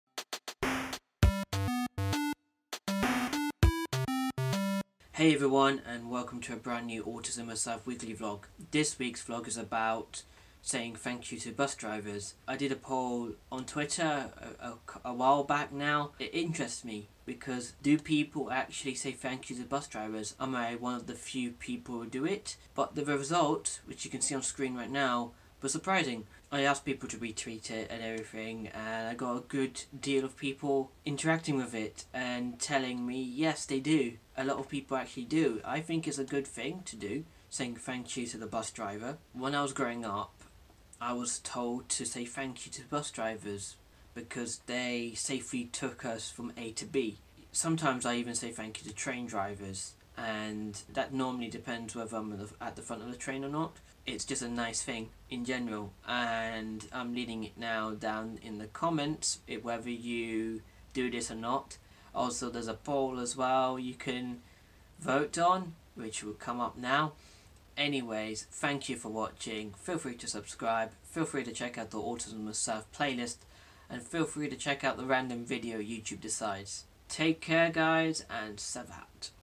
Do People Say Thank You To The Bus Driver? | AutismWithSav Weekly Vlog Series
In this week's vlog I talk about thanking the bus driver. I also did a Twitter Poll about this and the results surprised me. Let me know down below in the comments and poll whether you do this too?